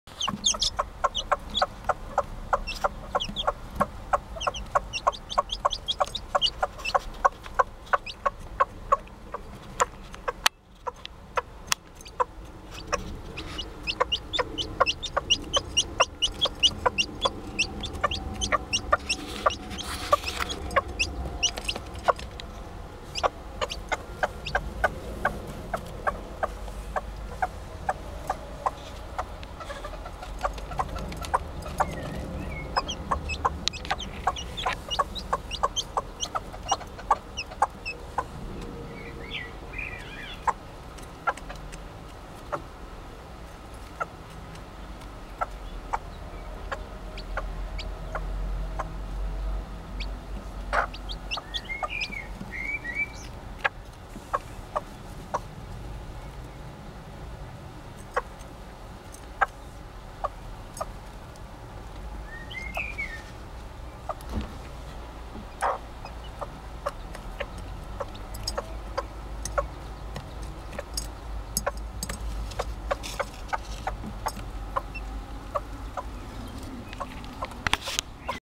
Quasimodo.AVI
Sable Poot.
Heres my Serama looking after her new chick. She dont know it yet but its a Sable Poot chick.
Damn these boots. I cant walk properly...or climb anything for that matter.